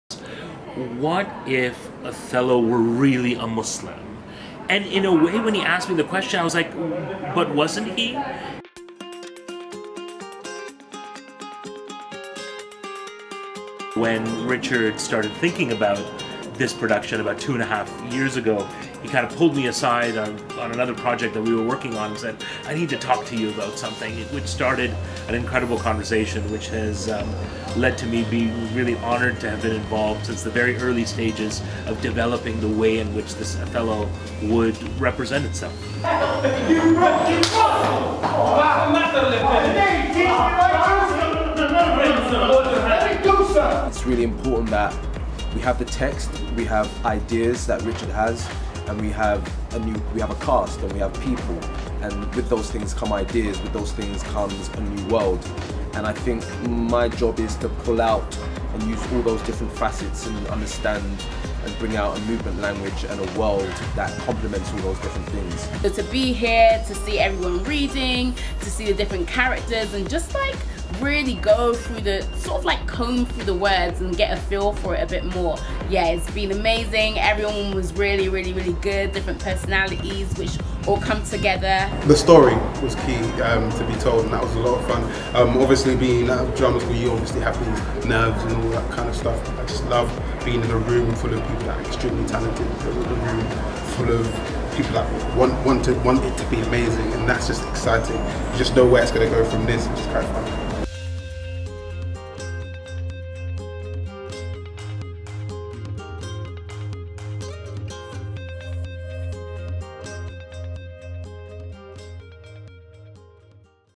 Othello | 2018 Behind the Scenes
ETT's Othello returns with a second run, with Victor Oshin in the title role. Directed by Richard Twyman and co-produced by English Touring Theatre, Shakespeare at the Tobacco Factory and Tobacco Factory Theatres, the show will hit UK cities including Oxford, Poole, Doncaster and Coventry.

Check out this behind the scenes clip for more about the show.

Filmed by Siana Bangura - Digital Content Producer
Edited by Troy James Aidoo - Videographer & Director